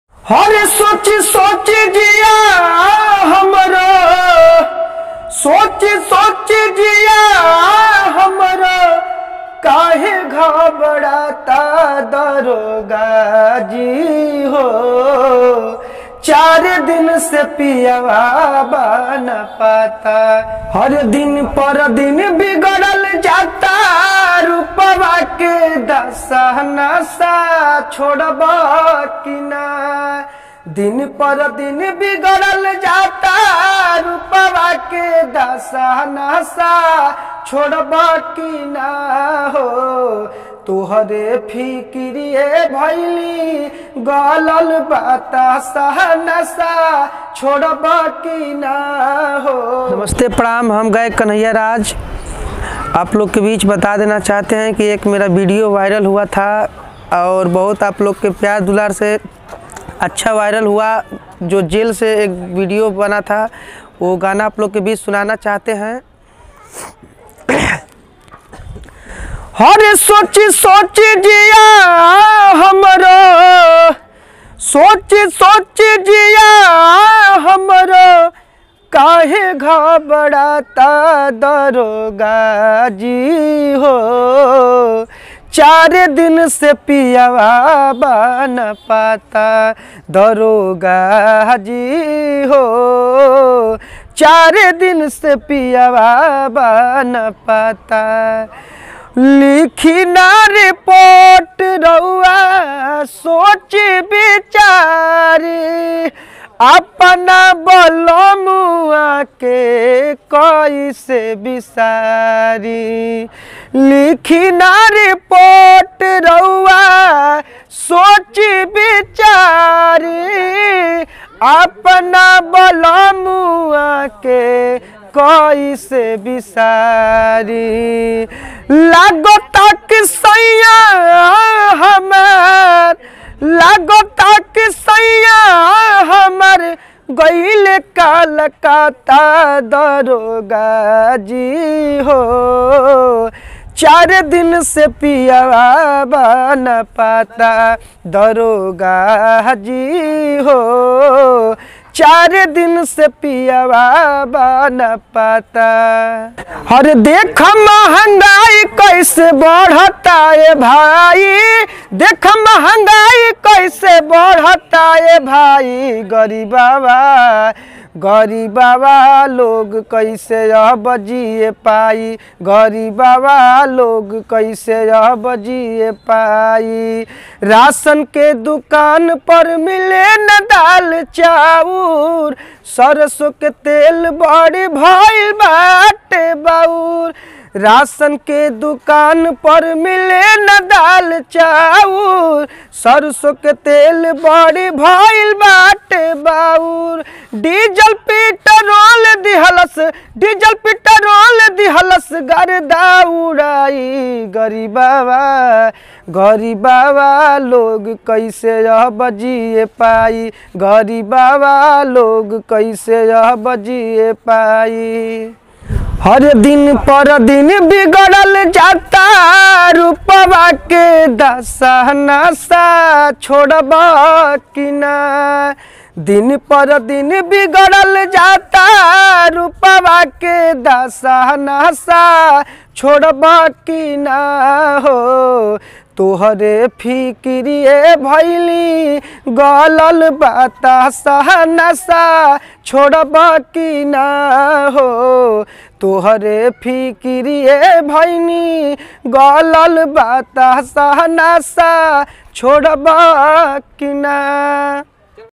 दरोगा जी के सामने कैदी ने गाया ऐसा गाना रातों-रात हो गया वायरल! Viral kanhaiya raj Singer
दरोगा जी के सामने गाया ऐसा गाना रातों-रात हो गया वायरल